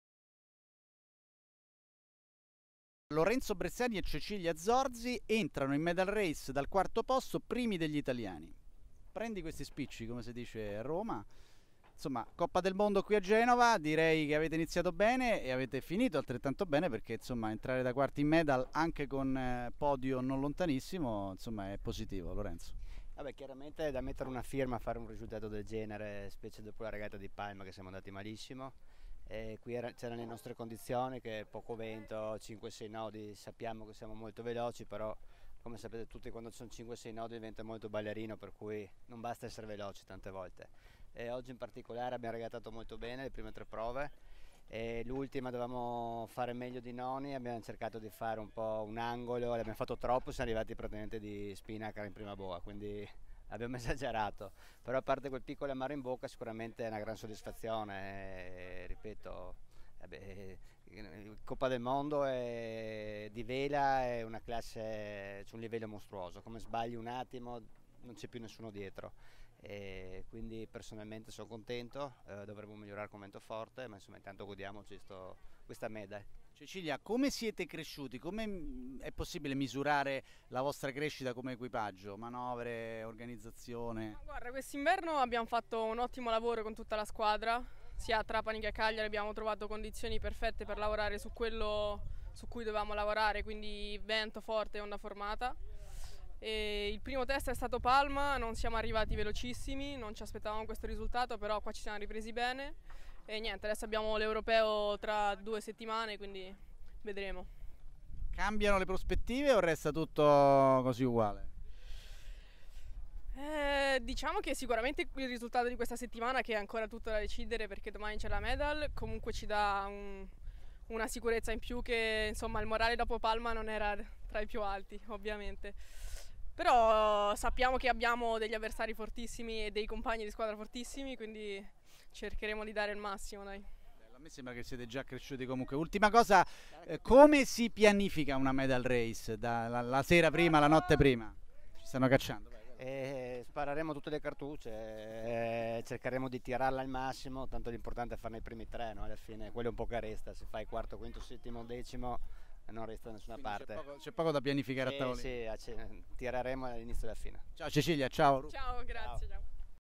Genova, Rufo e Cecilia non mollano mai
Coppa del Mondo di vela olimpica, condizioni di vento leggero a Genova, e Lorenzo Bressani con Cecilia Zorzi vanno forte. Sono quarti prima della Medal Race, a pochi punti dal podio. Ecco Rufo e Cecilia al rientro dopo una grande giornata